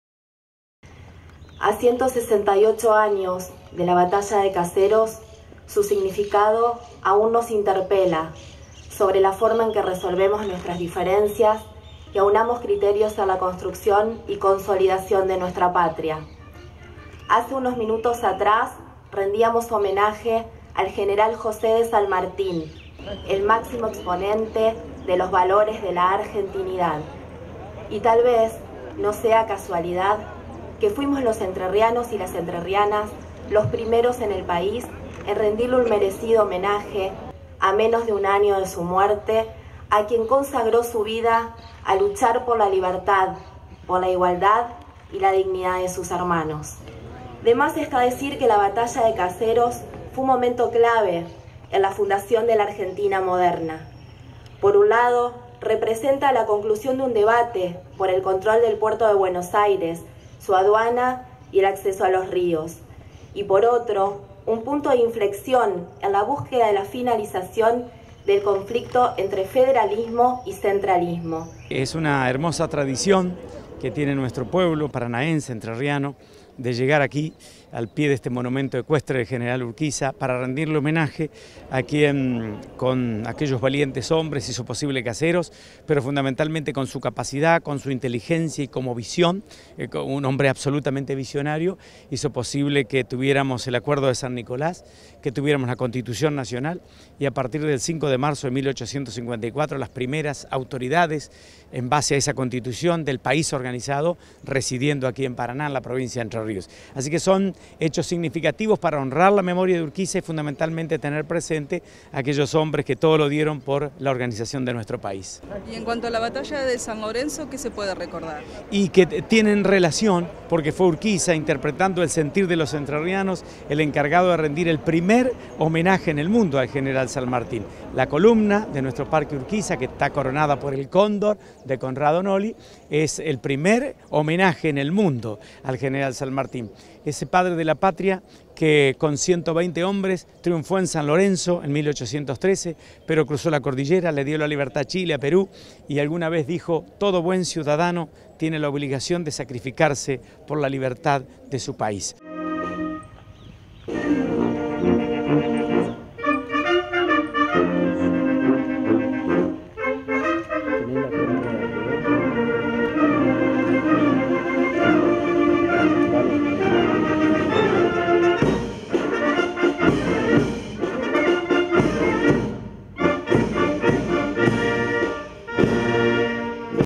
Realizaron el acto por la batalla de Caseros en el monumento a Urquiza
A 168 años de la batalla de Caseros, se realizó en Paraná, un acto en el monumento de Justo José de Urquiza, ubicado en el parque que lleva su nombre.